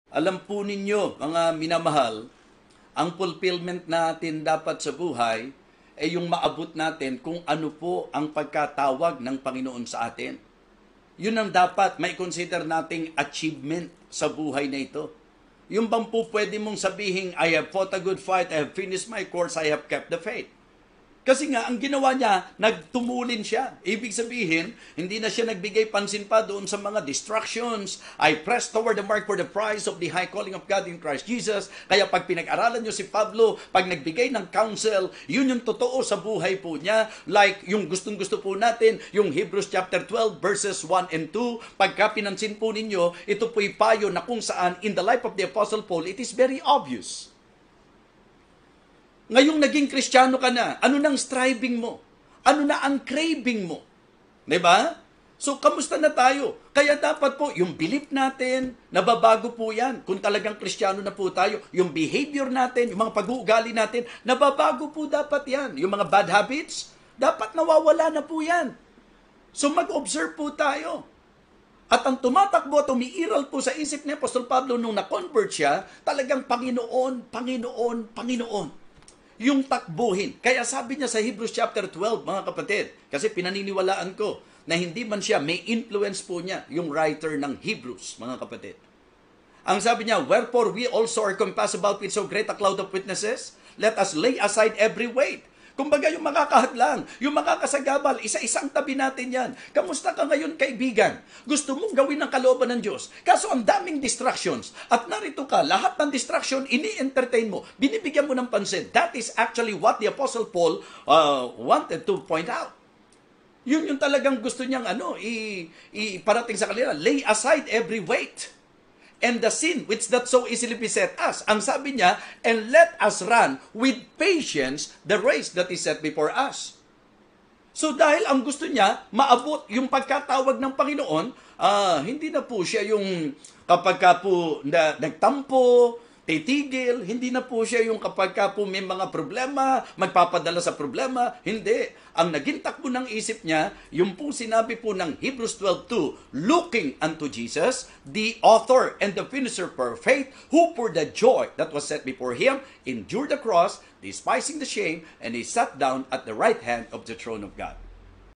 Looking Unto Jesus The Author And Finisher Of Our Faith - Bishop Freddie De Vega
Looking unto Jesus the author and finisher of our faith; who for the joy that was set before him endured the cross, despising the shame, and is set down at the right hand of the throne of God.

Hebrews 12:2